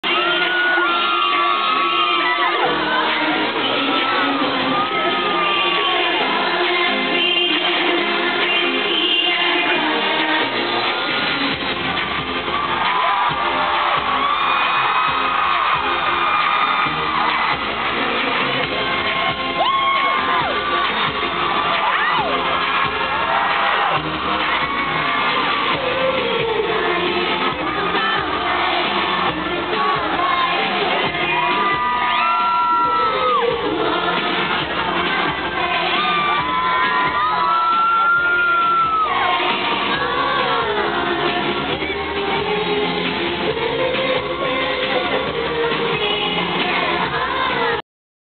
Britney Spears - Femme Fatale Tour - 3 - Sacramento - 6/16/2011
Femme Fatale Tour at Power
Balance Pavilion in Sacramento, California, June 16, 2011.